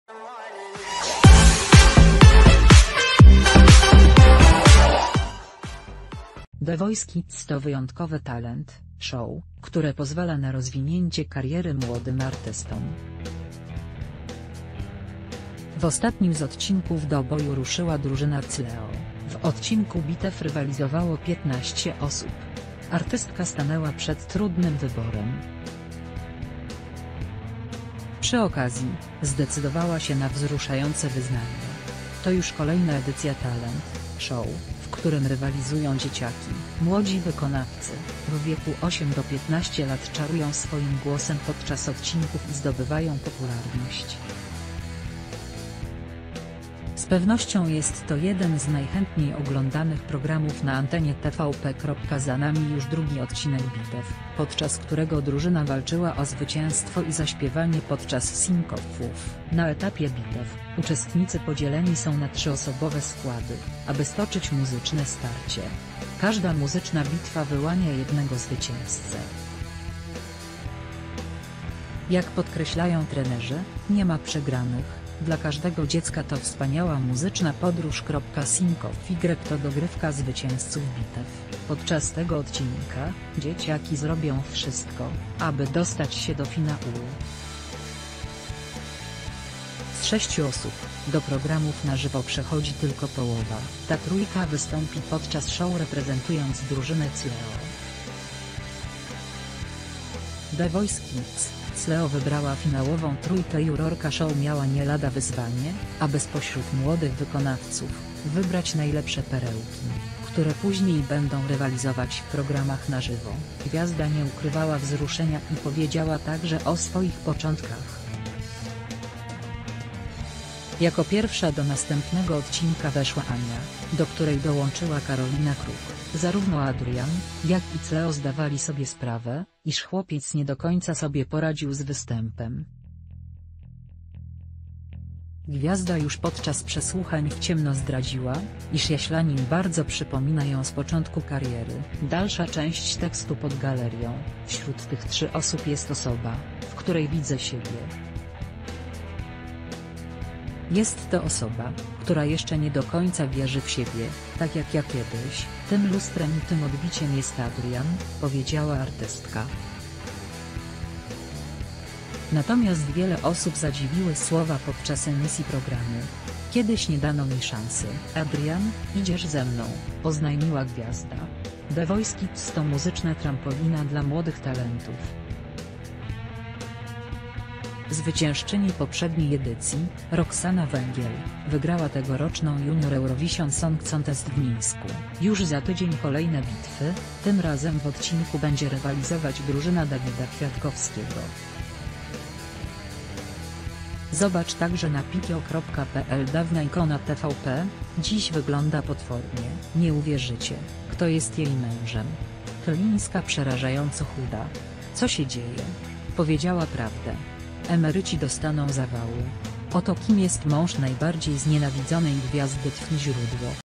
„Mi kiedyś szansy nie dano”. Wruszające słowa Cleo w The Voice Kids - Aktualności 24/7
The Voice Kids to wyjątkowe talent- show, które pozwala na rozwinięcie kariery młodym artystom. W ostatnim z odcinków do boju ruszyła drużyna Cleo. W odcinku Bitew rywalizowało 15 osób. Artystka stanęła przed trudnym wyborem. Przy okazji, zdecydowała się na wzruszające wyznanie. to już kolejna edycj...